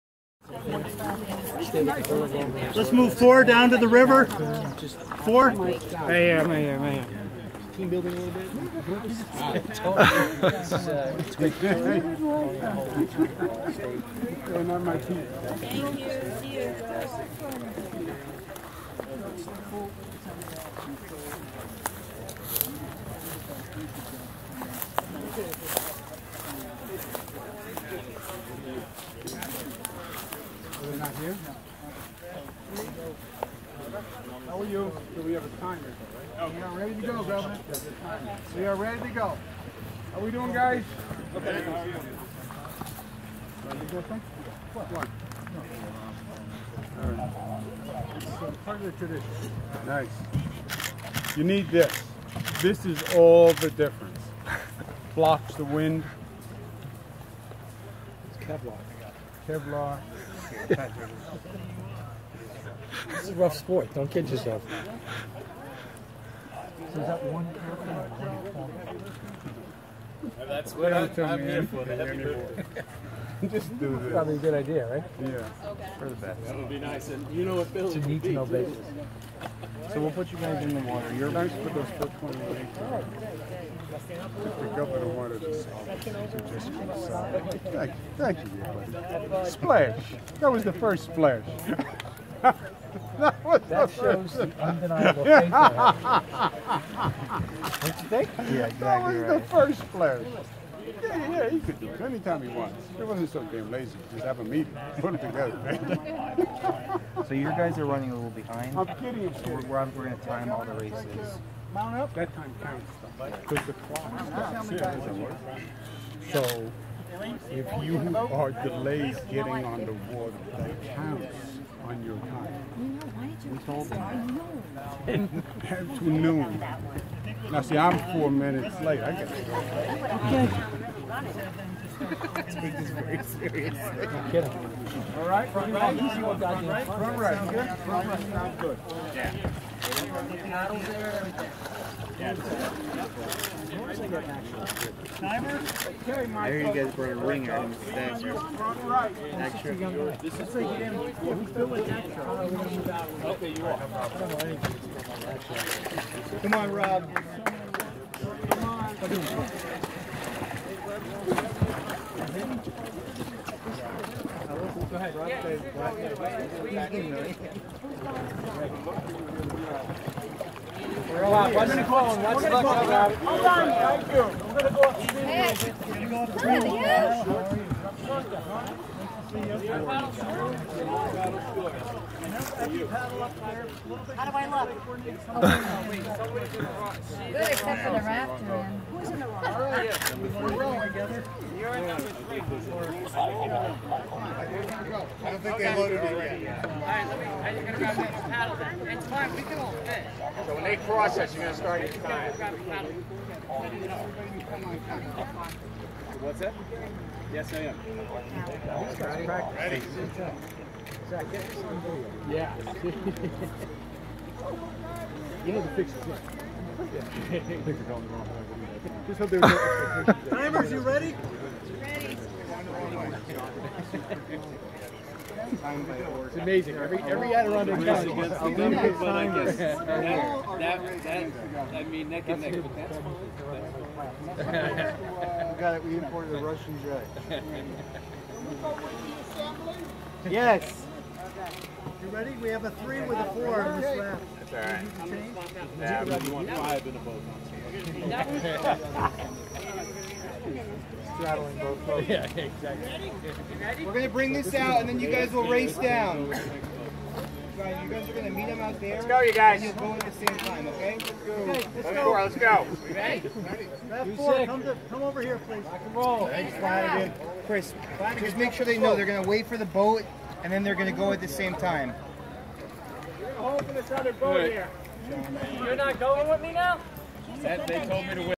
Governor takes to Delaware rafting
On June 30, 2016, NYS Governor Andrew Cuomo raced other state and local government leaders as part of his Catskill Summer Challenge. They launched into the Delaware River near the Roebling Bridge in Minisink Ford.